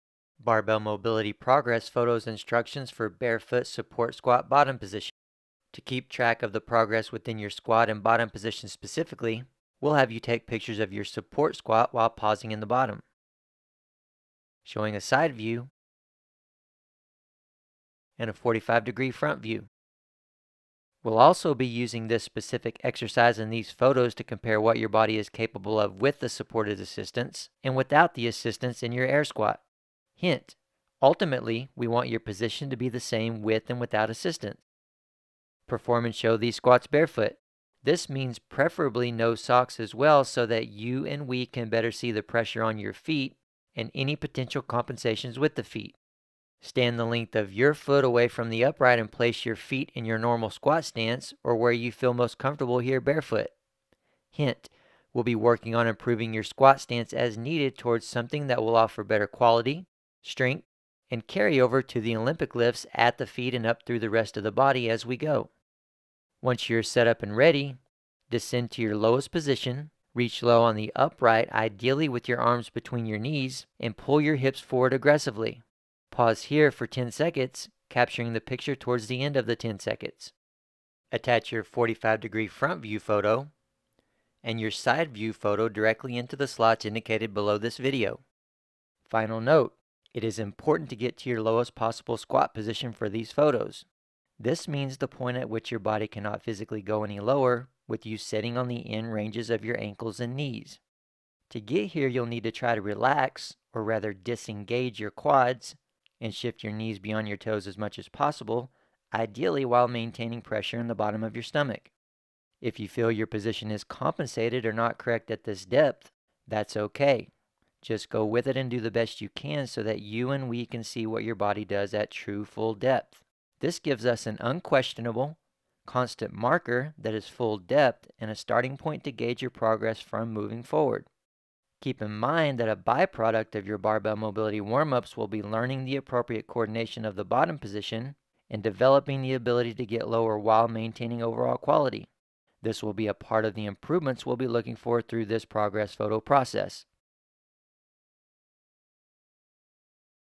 Barbell Mobility Progress Photos Instructions: Barefoot Support Squat Bottom Position
To keep track of the progress within your squat and bottom position specifically, we’ll have you take pictures of your support squat while pausing in the bottom, showing a side and 45 degree view. We’ll also be using this specific exercise and these photos to compare what your body is capable of WITH the supported assistance and without the assistance in your air squat (HINT: ultimately we want your position to be the same with and without assistance).

1. Perform and show these squats barefoot (this means preferably no socks as well so that you and we can better see the pressure on your feet and any potential compensations with the feet).
2. Stand the length of your foot away from and upright and place your feet in your normal squat stance or where you feel most comfortable here barefoot (HINT: we’ll be working on improving your squat stance as needed towards something that will offer better quality, strength, and carryover to the Olympic lifts at the feet and up through the rest of the body as we go).
3. Once you are set up and ready, descend to your lowest position, reach low on the upright ideally with your arms between your knees, and pull your hips forward aggressively. Pause here for 10 seconds, capturing the picture towards the end of the 10 seconds.
4. Attach your side view photo, and your 45 degree front view photo directly into the slots indicated below this video.

NOTE: It is important to get to your lowest possible squat position for these photos. This means the point at which your body cannot physically go any lower with you sitting on the end ranges of your ankles and knees. To get here you’ll need to try to relax or disengage your quads and shift your knees beyond your toes as much as possible, ideally WHILE MAINTAINING pressure in the bottom of your stomach. If you feel that your position is compensated or not correct at this depth, that's ok, just go with it and do the best you can so that you/we can see what your body does at true full depth. This gives us an unquestionable, constant marker (full depth) and a starting point to gauge your progress from moving forward. Keep in mind that a by-product of your Barbell Mobility warmups will be learning and developing the ability to get lower WHILE maintaining overall quality; this will be a part of the progress we’ll be looking for in the follow up photos.